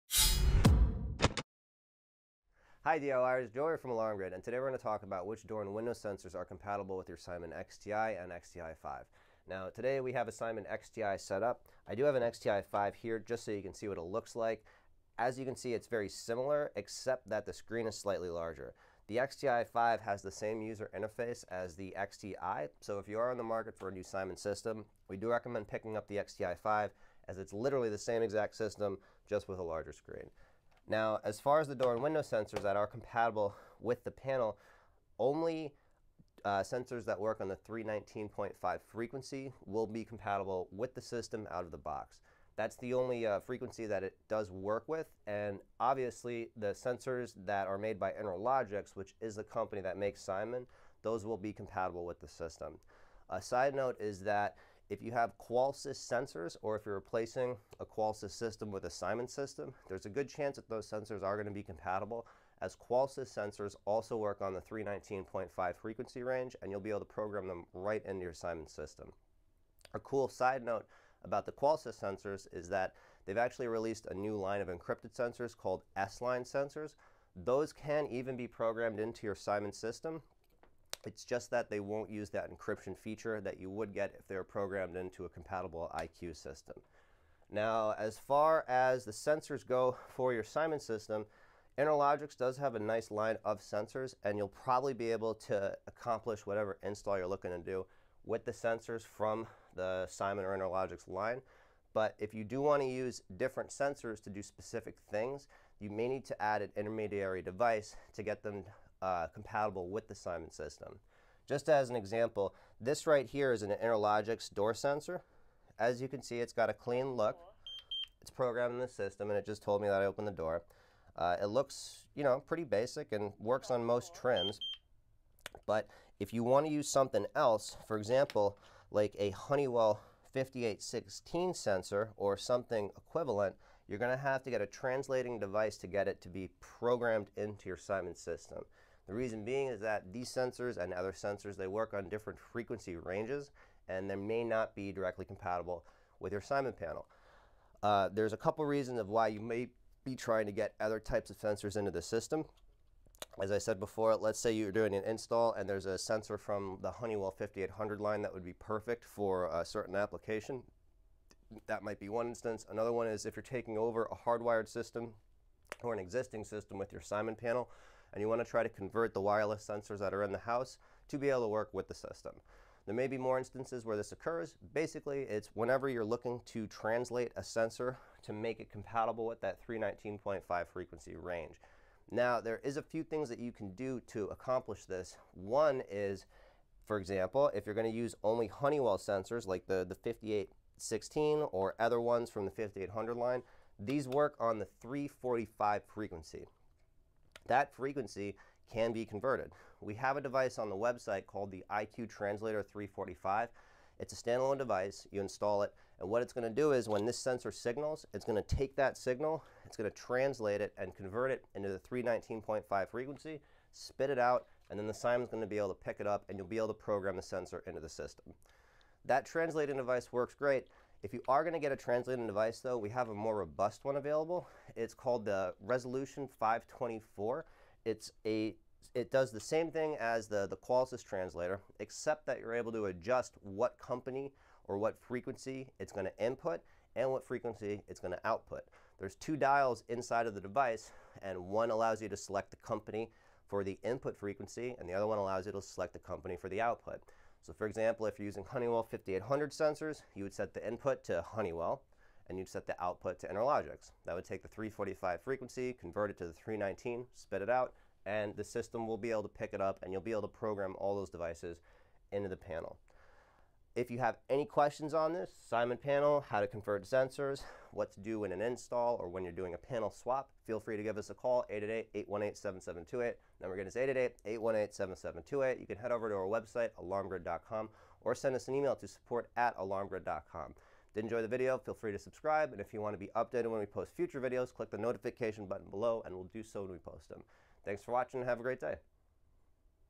Door Sensors that Work on the Interlogix Simon XTi & XTi-5
In this video, Joe discusses the door and window contacts that can be used with the Interlogix Simon XTi and Simon XTi-5 Alarm Systems. In most cases, a user will use wireless contacts with these systems. However, the sensors must communicate at the 319.5 MHz frequency that is used by these systems.

Door and window contacts are perhaps the most important sensors used by an alarm system. These sensors let the system know whenever a door or window has been opened. Although it is technically possible to set up a complete security system without door and window contacts, there's very little reason to try and do this. Having the system know when a door or window is opened is one of the biggest benefits to owning a security system. A user can also set up the system to trigger an alarm if a door or window is opened while the system is armed and it is not disarmed within a predetermined entry delay period. This is commonly referred to as an "entry/exit" zone, and it is one of the most commonly used zone types. Another commonly used zone type for doors is the "interior" zone type. This zone type will cause an immediate alarm if the sensor is activated while the system is set to arm away. However an interior zone will be inactive when the system is set to arm stay.

The Interlogix Simon XTi and Simon XTi-5 Alarm Systems both include a wireless receiver that supports sensors at the 319.5 MHz frequency. If the wireless door and window contact does not operate at the 319.5 MHz frequency, then it will not be supported by the system, and it cannot be used. Door and window contacts that work at 319.5 MHz include those from Qolsys and Interlogix/GE. Additionally, a user can used hardwired contacts in one of two ways. These systems have two hardwired terminals on the back for supporting two hardwired door and window contacts. A user can also use a compatible wired to wireless converter, like the Qolsys IQ Hardwire-16, to add additional contacts.